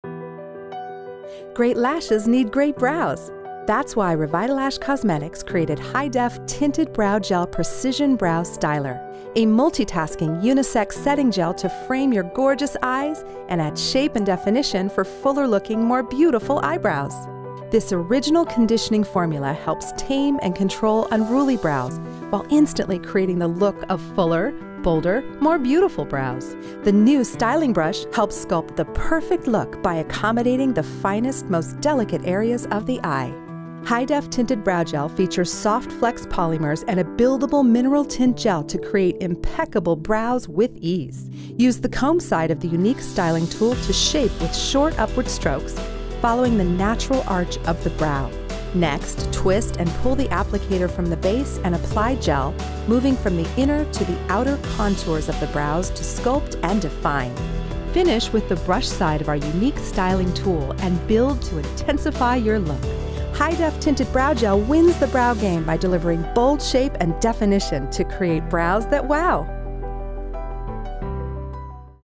Revitalash HI-DEF BROWGEL
Een multi-tasking, unisex, wenkbrauwgel die helpt bij het shapenvan de wenkbrauwen.
Wat doet het:onze originele verzorgende wenkbrauwgel, geeft niet alleen een subtiele en natuurlijke kleur aan de wenkbrauwen, maar hydrateert en conditioneert de wenkbrauwhaartjes zodat ze gezonder en voller ogen. Kan dunnere plekken eenvoudig opvullen dankzij de minerale kleurgel die een subtiele kleur afgeeft voor gedefinieerde, mooie wenkbrauwen. Met de wenkbrauwborstel kunt u de wenkbrauwgel goed verdelen over de haren voor een optimaal resultaat.